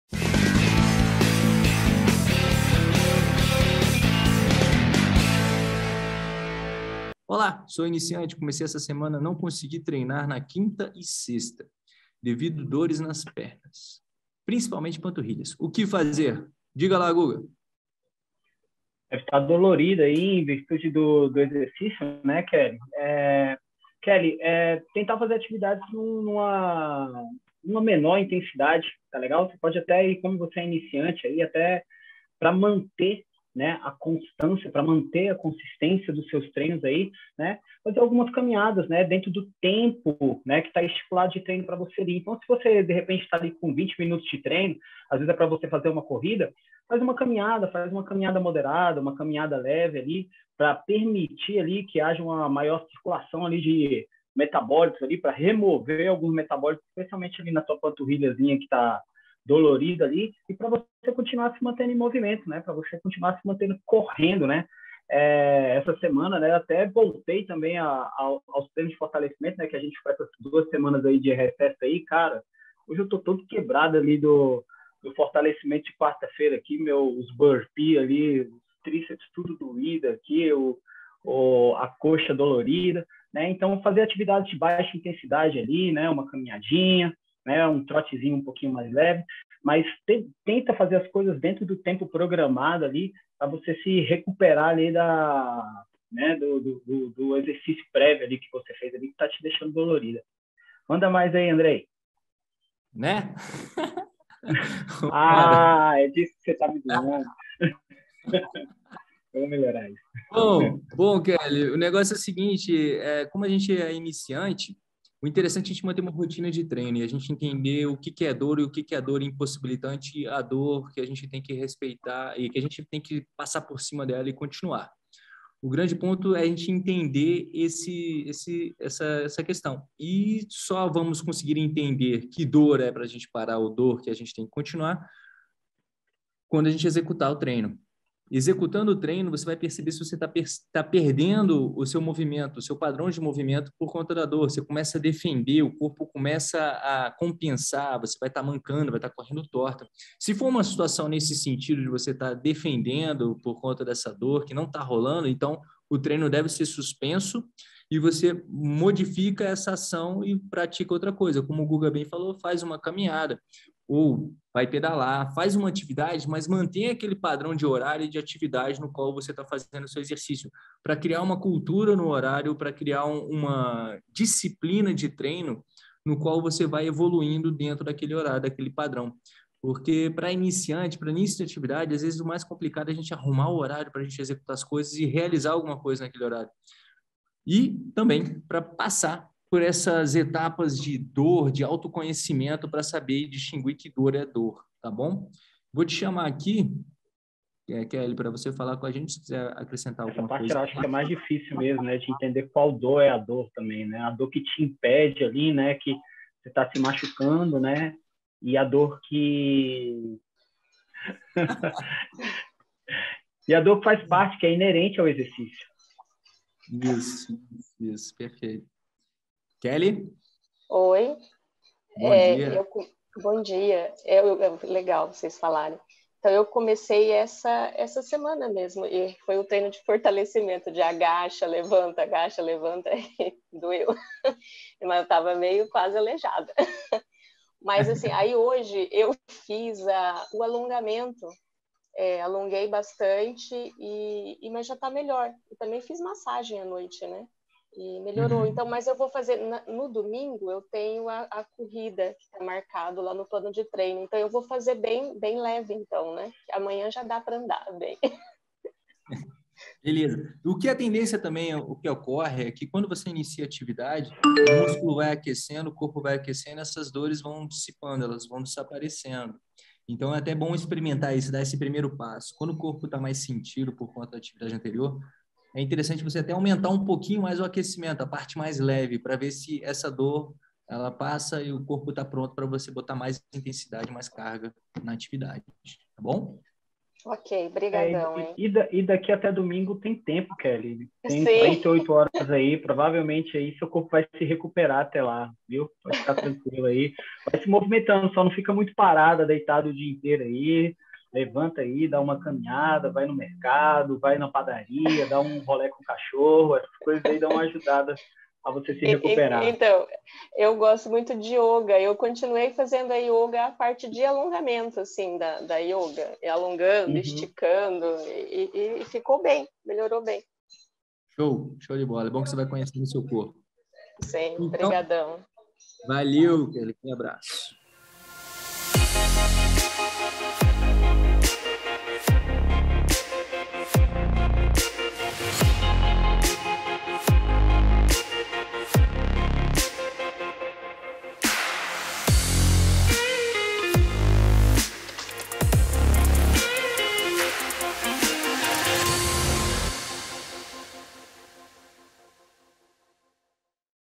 O QUE FAZER quando os músculos ficarem DOLORIDOS pós-corrida? 💪
Quem nunca sofreu com as pernas doloridas nos primeiros treinos, que atire o primeiro tênis!!! Ou então correu com mais intensidade em um dia, mas no outro não conseguiu nem andar direito... Se você já passou por alguma dessas situações, vai adorar esse corte de um dos nosso plantões de tira-dúvidas exclusivo para alunos. Neste vídeo, o nosso coach Andrei Achcar e o nosso treinador Gustavo Guedes vão te dar dicas do que você pode (ou não) fazer quando isso acontecer. Assista até o final para não perder nenhuma dica 👀

Quer ter resultados na corrida e com saúde? Treine com a metodologia mais completa do mercado »